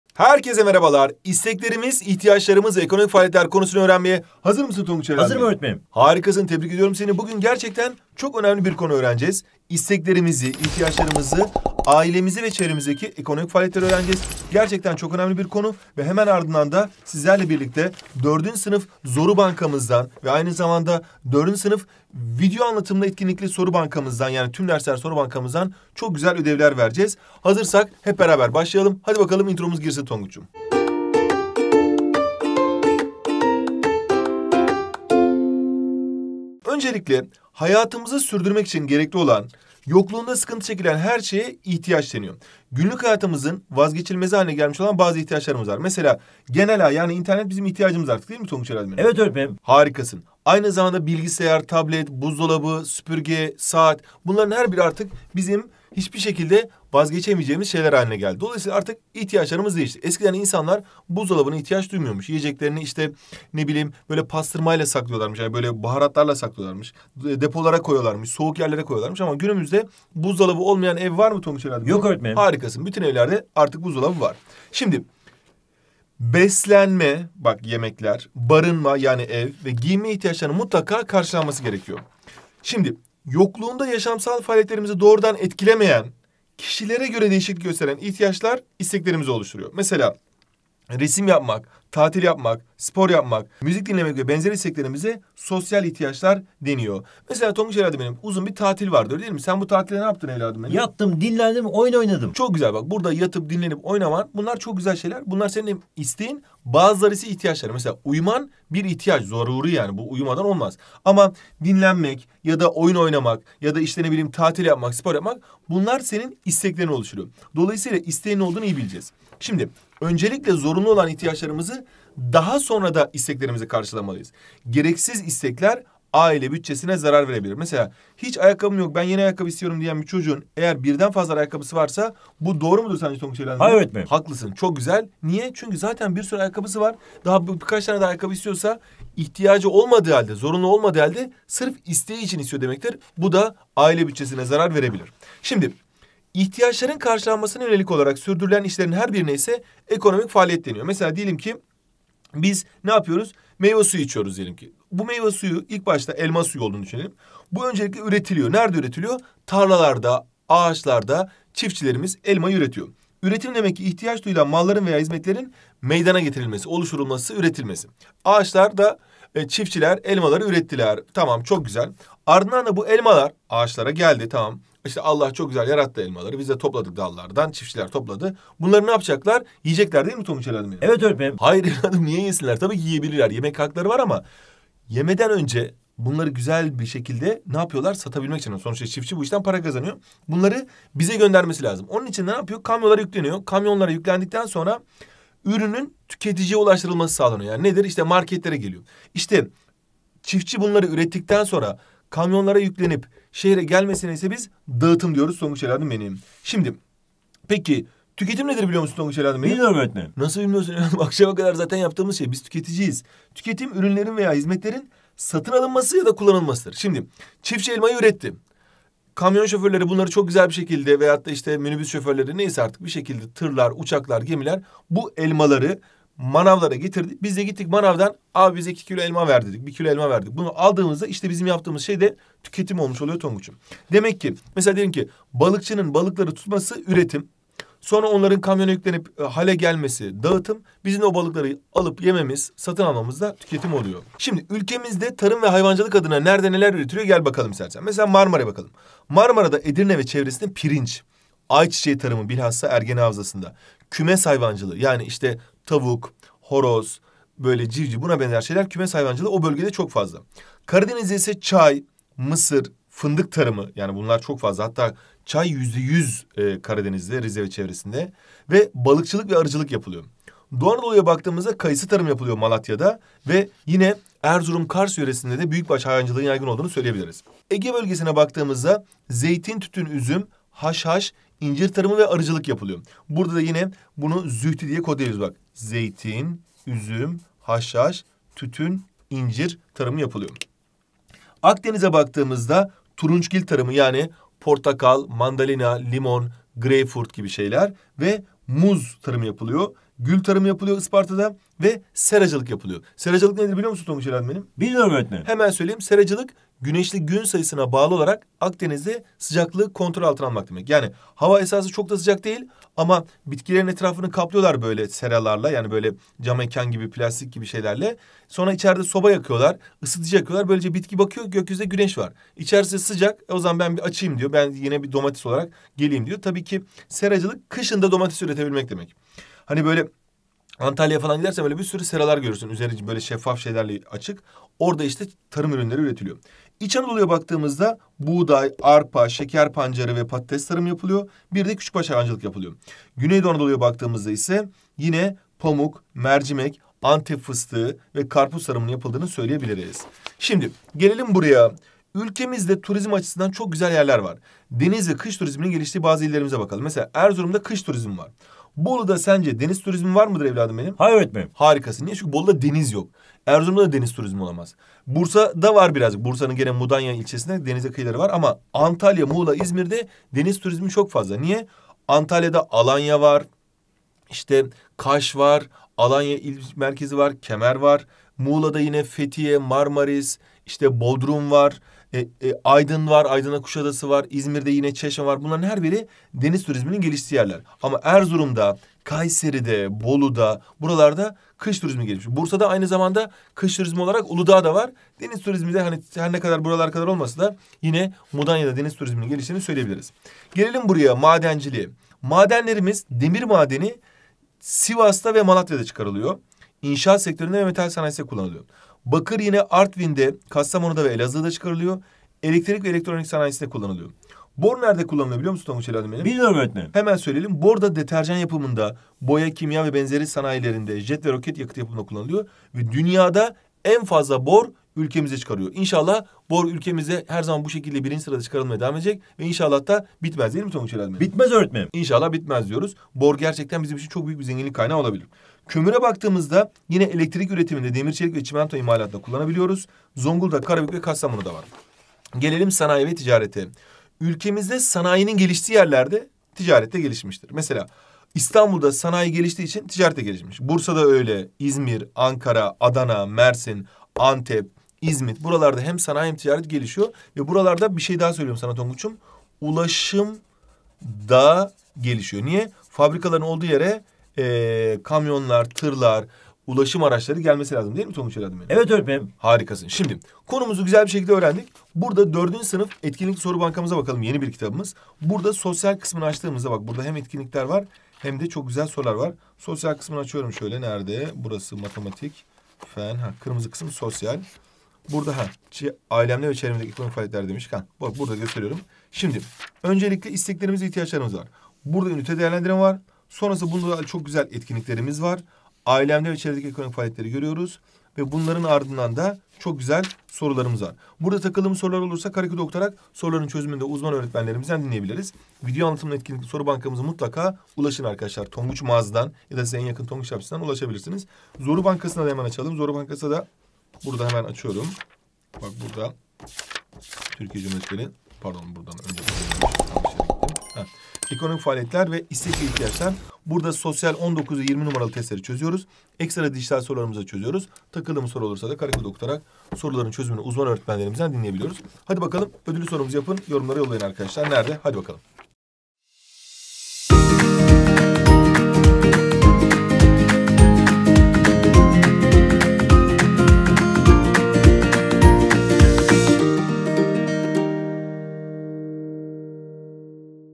İsteklerimiz, İhtiyaçlarımız ve Ekonomik Faaliyetler 👕 4.Sınıf Sosyal #2023
✅ Etkinliklerle Konuları En Eğlenceli Şekilde Pekiştir!
🎯 Sosyalde Yapamadığın Zor Soru Kalmasın!
✨Dönem boyunca; canlı yayınlar, konu anlatımları, soru çözümleri ve rehberlik videolarıyla, tonguç yanında! Bugün Sosyal dersimizin "İsteklerimiz, İhtiyaçlarımız ve Ekonomik Faaliyetler" konusunu işleyeceğiz. Hazırsan başlıyoruz! 😎